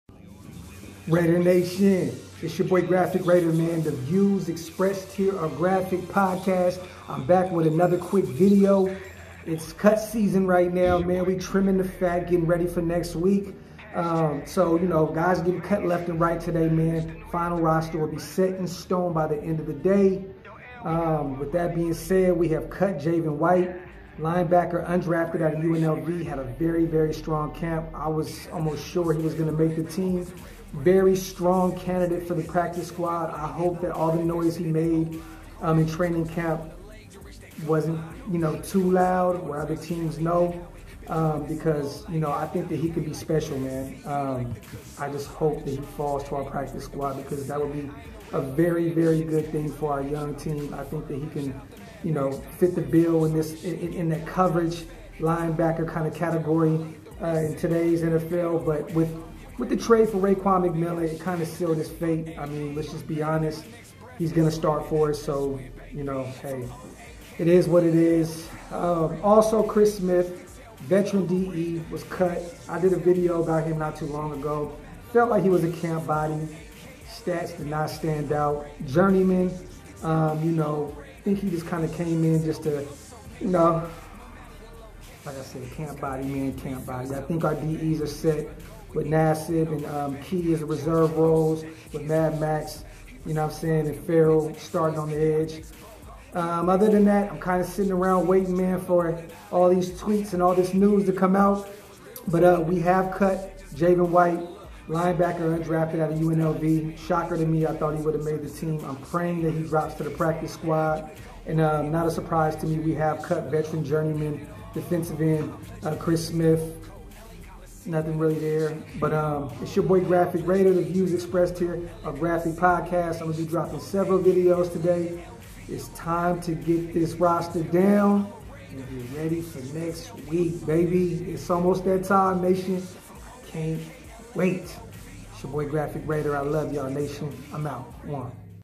Javin White CUT 🤯🔪 Chris Smith Axed ⚒ as well. Raiders Final Roster Will Be Set Today! 🏴‍☠️
The Views Expressed Here Are Graphk Podcast